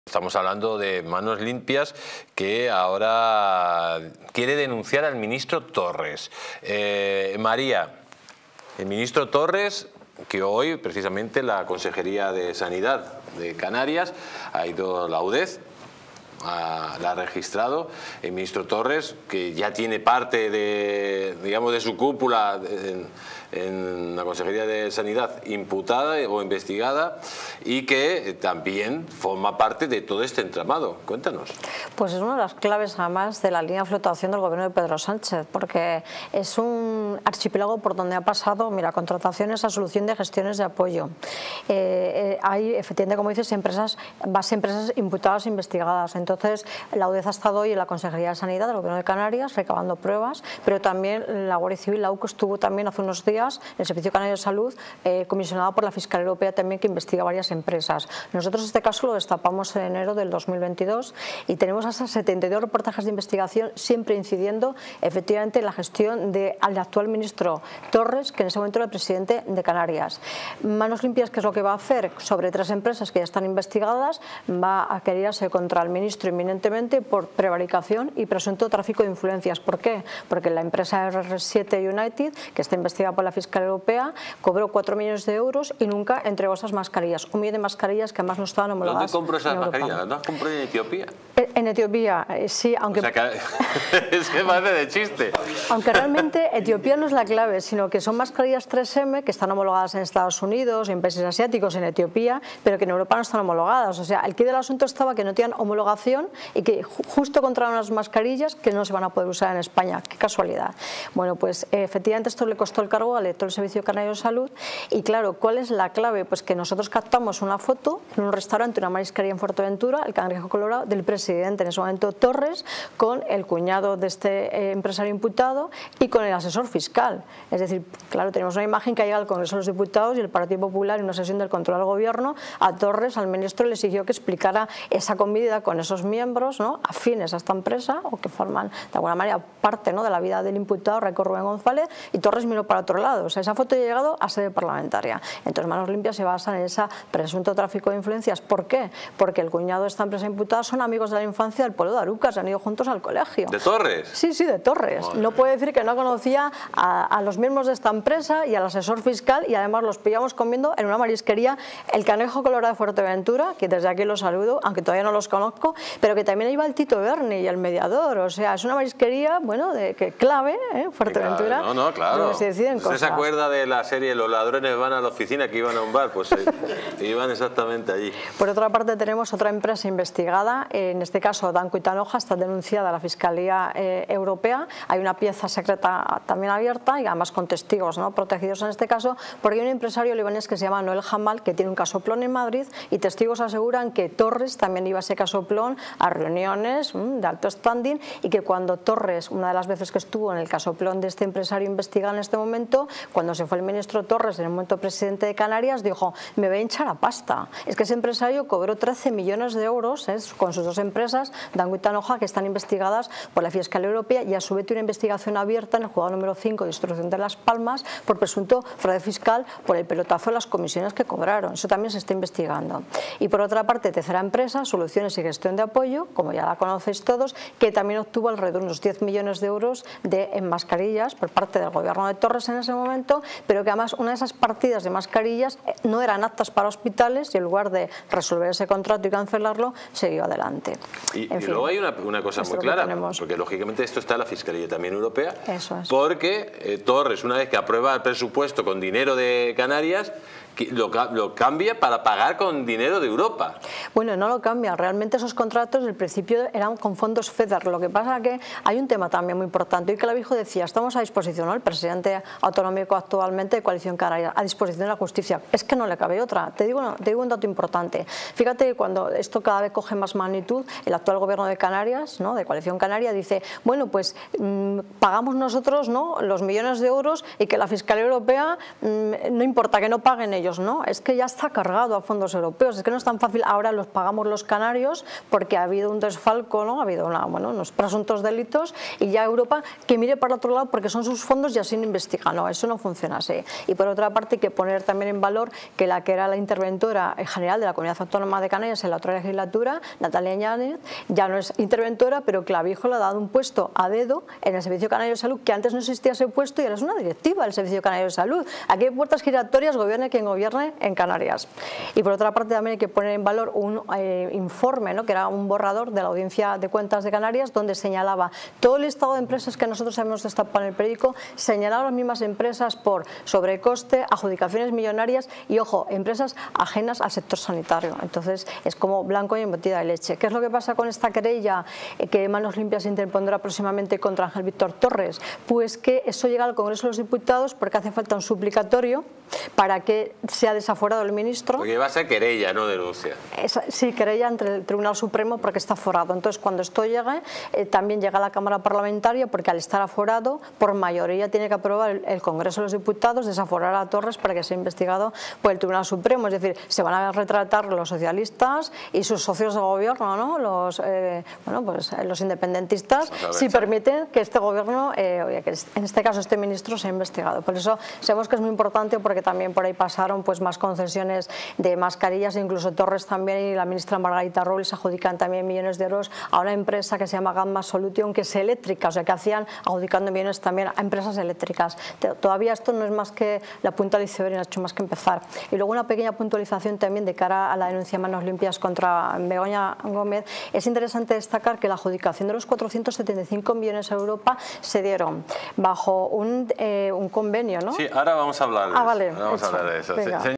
🔴BRUTAL🔴 el ministro Víctor Torres acusado de corrupción por millonarias compras de material Covid 🔴
En este video, descubre las impactantes acusaciones de corrupción contra Víctor Torres por la compra de material Covid falso. ¡No te lo pierdas en Distrito TV!